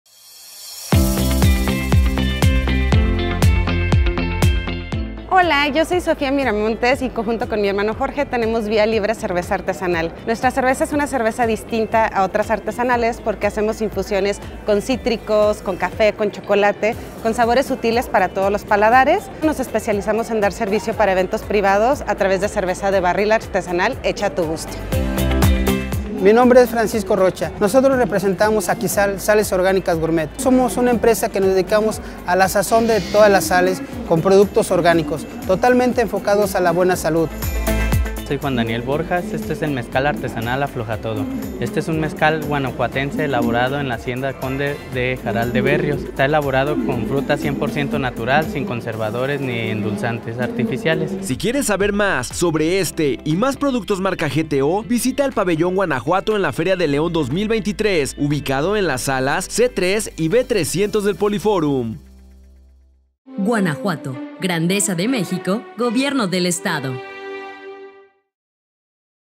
Agroalimentos en Pabellón Guanajuato 2023
Conoce algunos de los productos Marca Guanajuato que podrás adquirir en el Pabellón Guanajuato de la Feria de León 2023.

Visita PabellónGTO en la FeriaDeLeon, estaremos hasta el 7 de febrero 2023.😀